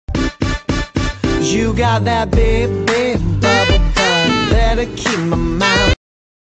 How to Slip On My Slippers Without Waking Him Up?
🔥 Want to start your day on the right foot? 🙌 In this amazing video, I'll show you a genius trick on how to slip on your cozy slippers without waking up your partner! 😴💤 Don't miss out on this life-changing hack! 💁‍♀️✨ Click here now! ⬇️✨